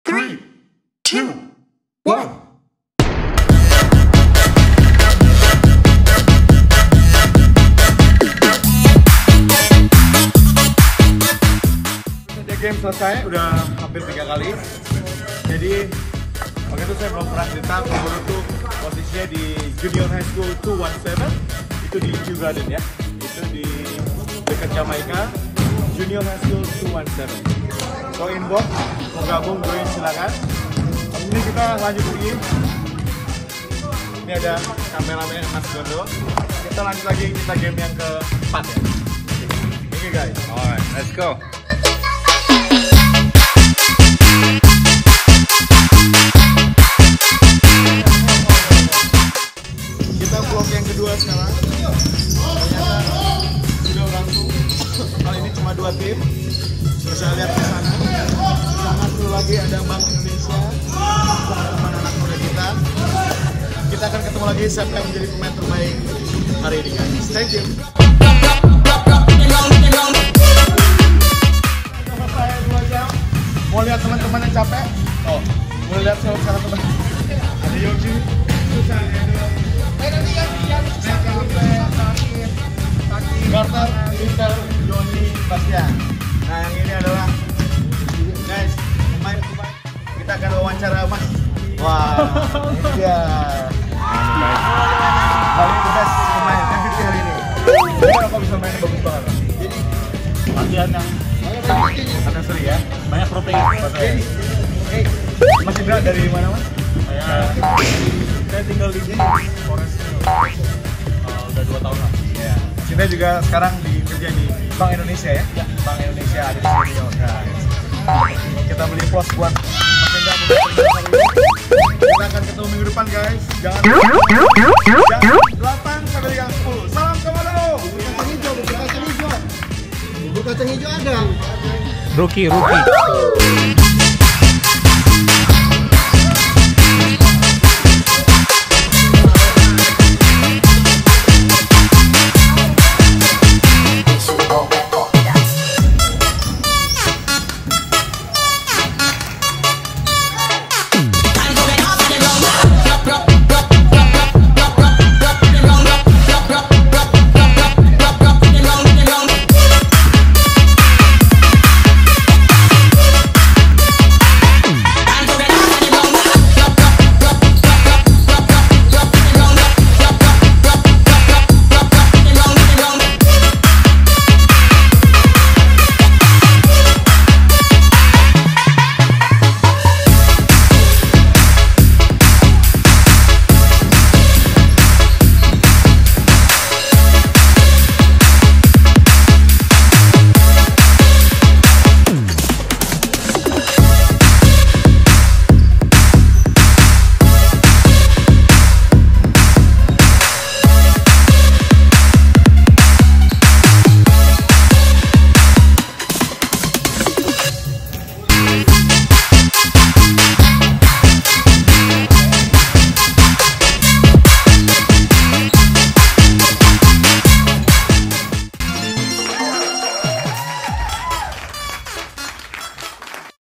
Komunitas basketball orang Indonesia vlog 2🇮🇩🏀💪
Komunitas Orang Indonesia di NY bermain basket untuk persatuan dan kesatuan bangsa gotong royong
Setiap hari Jumat pukul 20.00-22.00
Di JHS 217 Robert A Van Wyck Queens NY